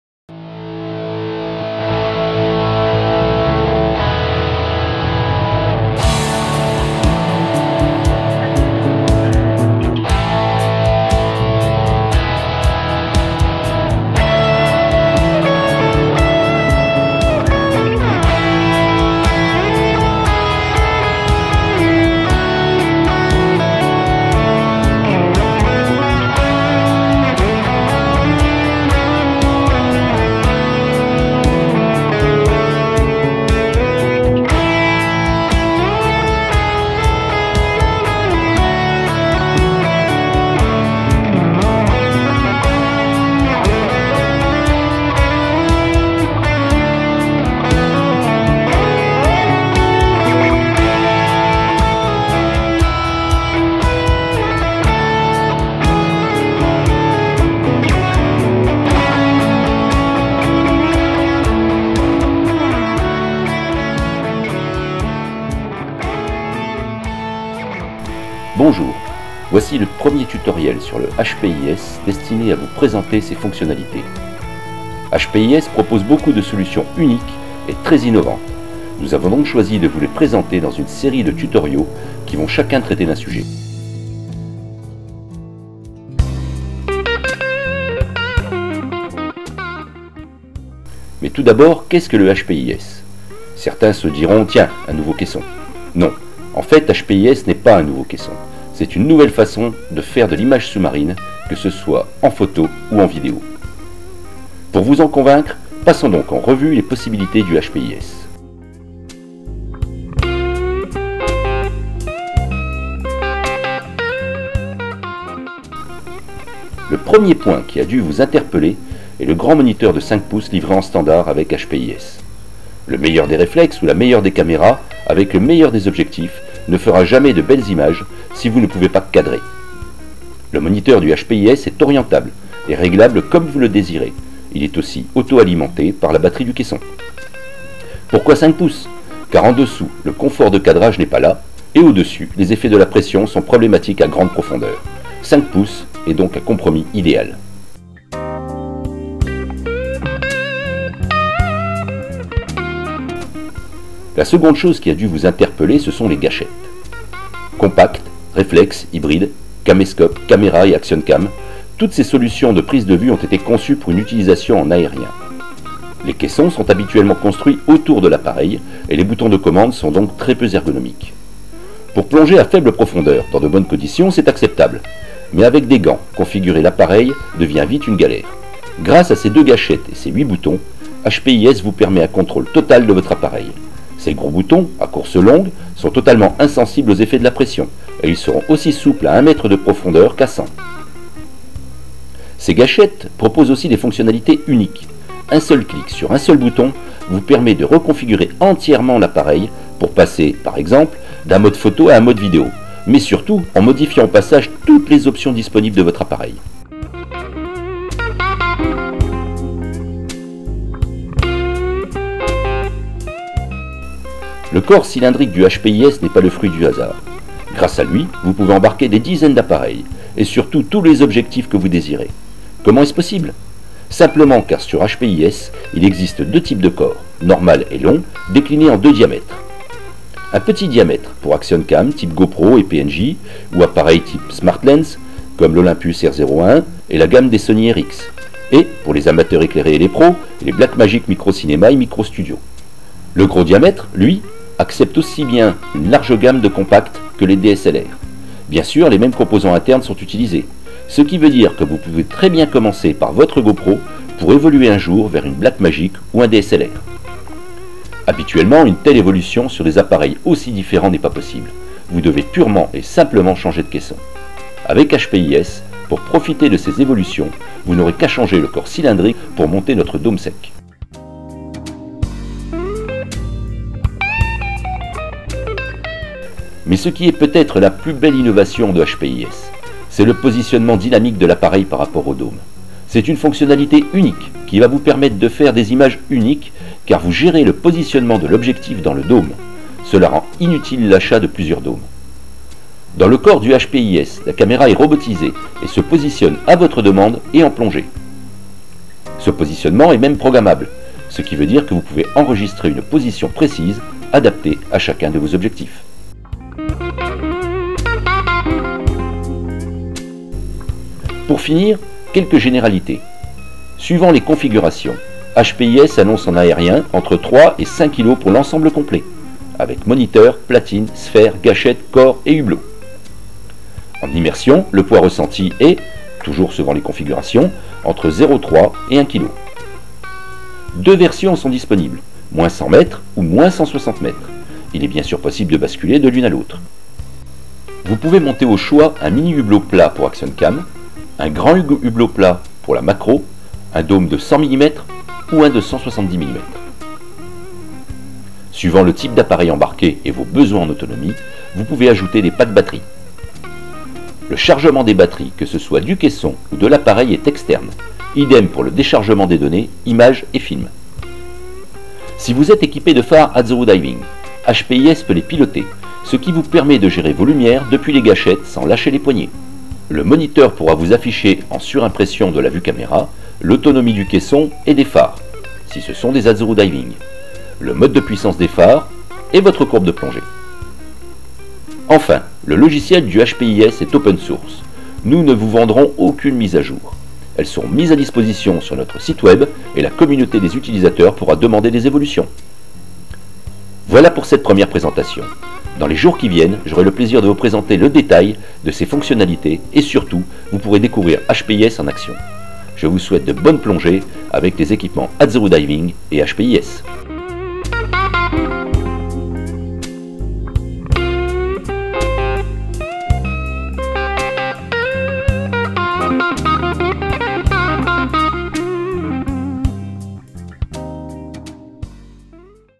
HPIS : Le concept
Ce tutoriel présente le système de prises de vues submersible HPIS d'Azuru Diving